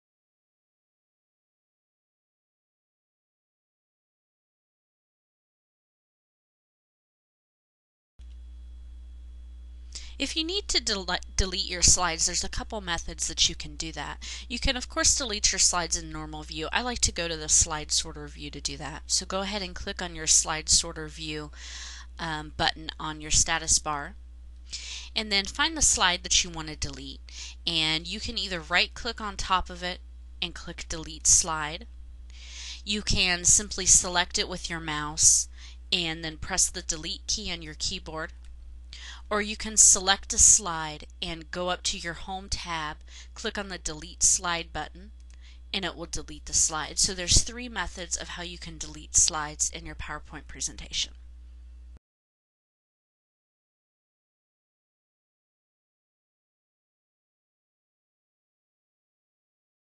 Delete Slides in PowerPoint 2007
See a higher quality video on TechTutor.TV! Slides can be easily deleted with your keyboard in Microsoft PowerPoint 2007's slide sorter view. In this video tutorial, you will learn how to delete slides in PowerPoint 2007. See higher quality tutorials all for FREE